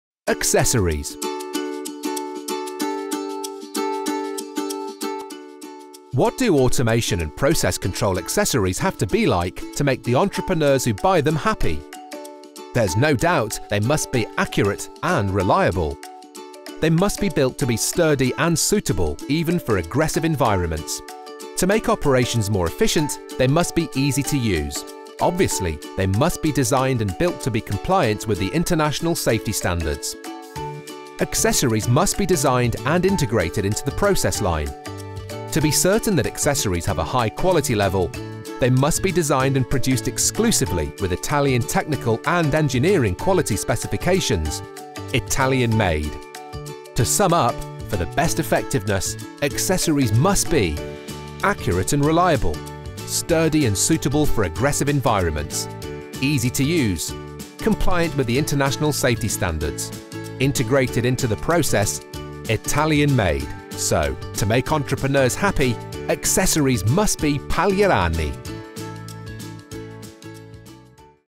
Accessories: empty bag feeder, full bag sealer, bag sewing machine, and quality control line - EN
A wide range of accessories for automating and controlling the bagging production process: empty bag feeder, full bag sealer, bag sewing machine, and quality control line.

Paglierani designs and builds a wide range of accessories for automating the bagging cycle (empty bag presentation), closing the full bag (sewing, welding and sealing machines), and quality control (metal detector, check weighing).

The manufacture philosophy and high level of know-how are the same applied for all of the machines produced by Paglierani.

A coherent, quality choice.